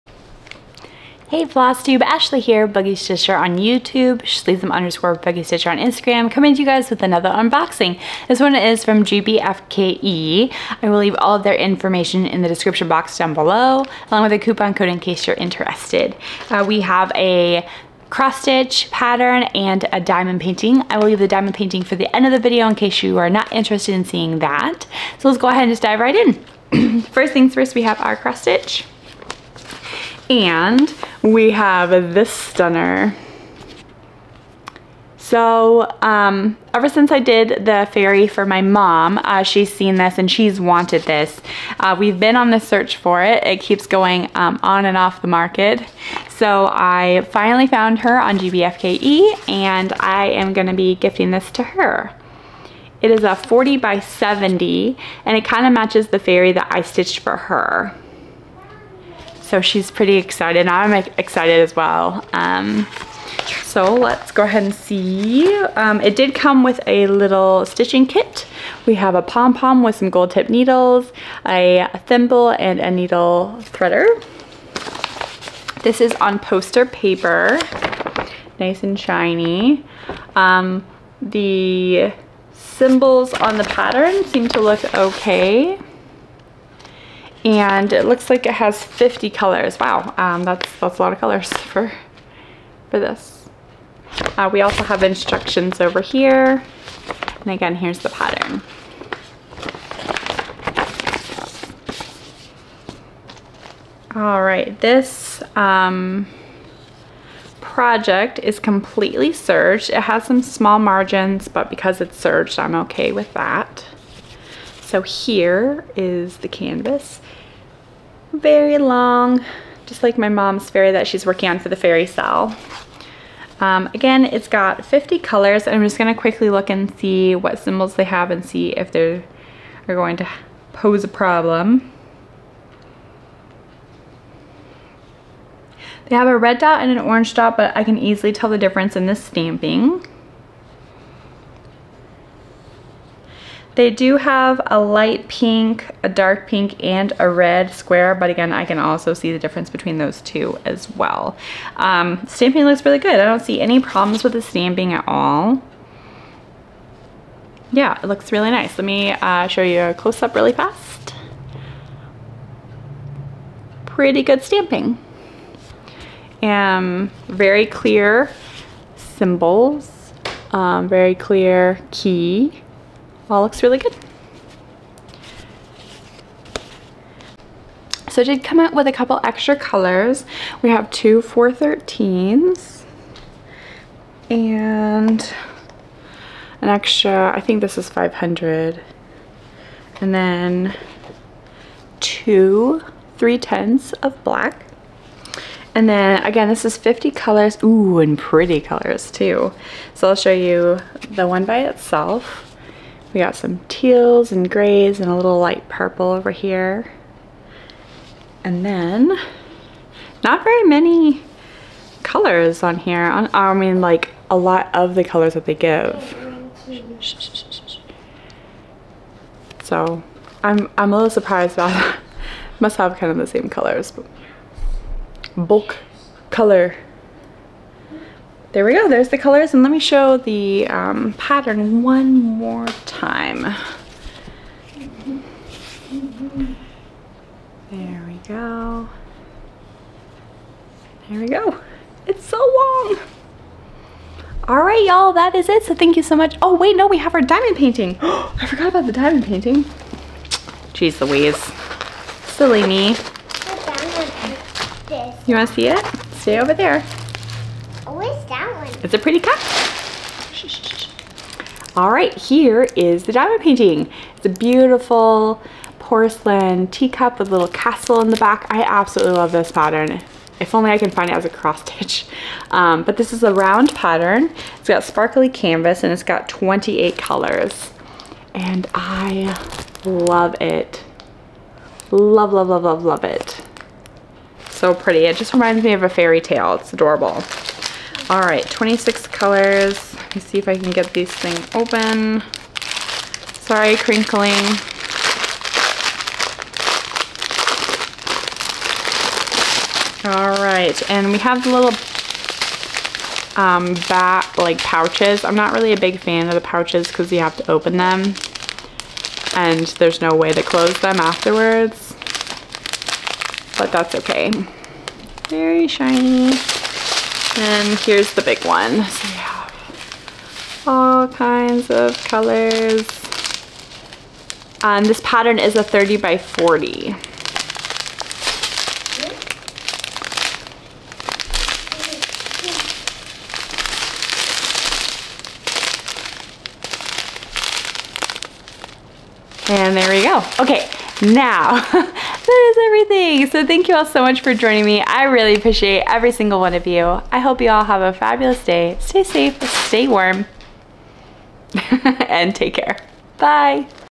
Flosstube #89 - Cross-Stitch Kit Unboxing and Review
Hey Everyone! Come hang out as we unbox some cross stitch kits from GBFKE! A cross stitch and diamond painting store. Check out their information below🎉

Boogie-03OFF

Products:
Fairy:

Diamond Painting:
Porcelain Cup Round: